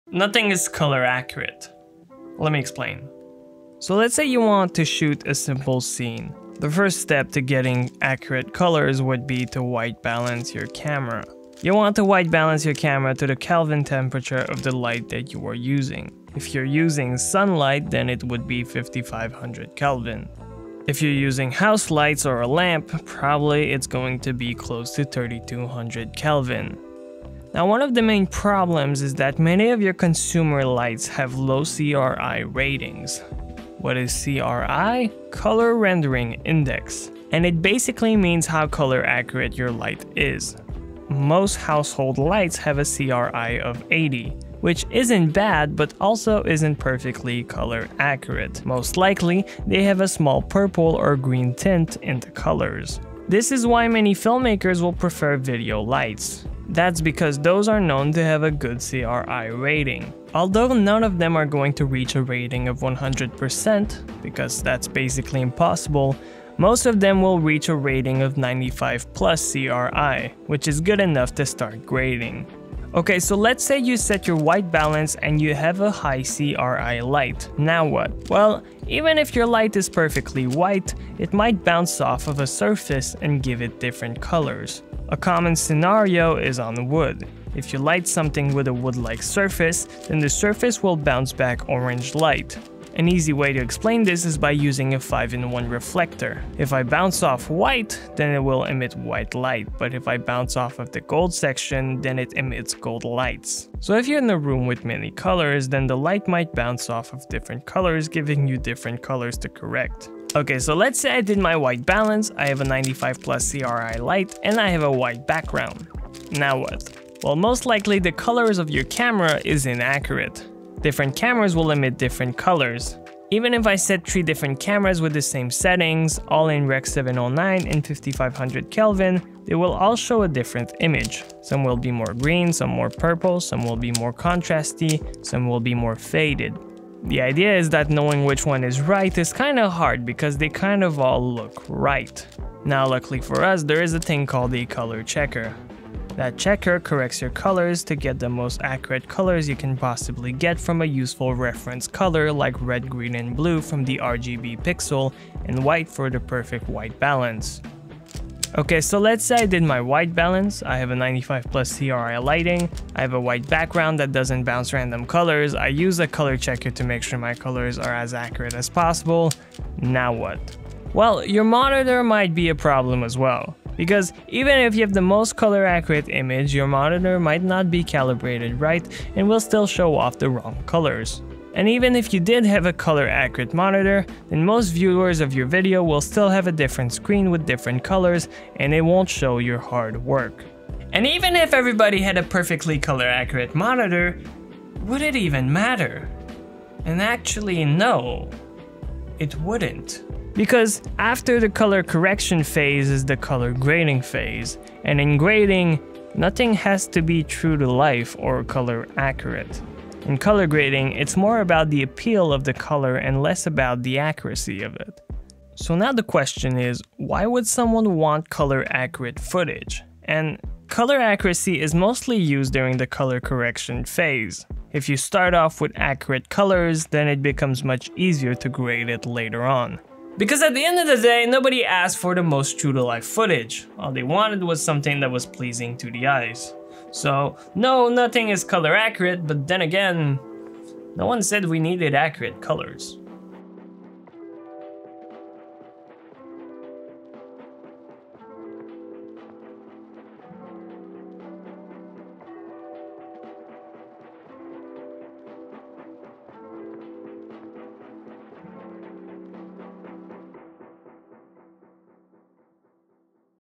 How the Accuracy of your COLORS is affecting your images.
Intro : 0:00
White Balance : 0:03
CRI : 0:27
Bounce : 1:12
Camera Color Science : 1:50
Monitor : 2:38
Does it even matter? : 3:10
Conclusion : 3:50